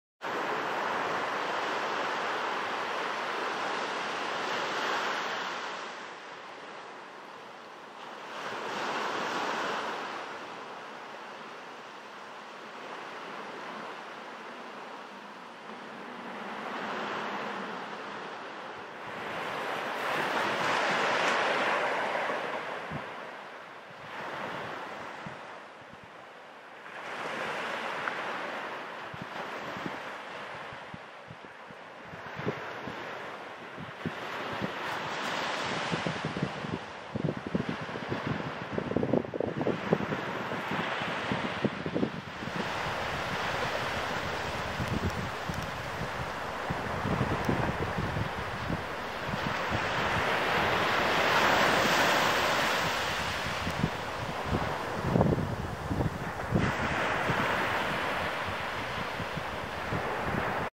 Sunset at Zandvoort
A winter sunset at Zandvoort, the Netherlands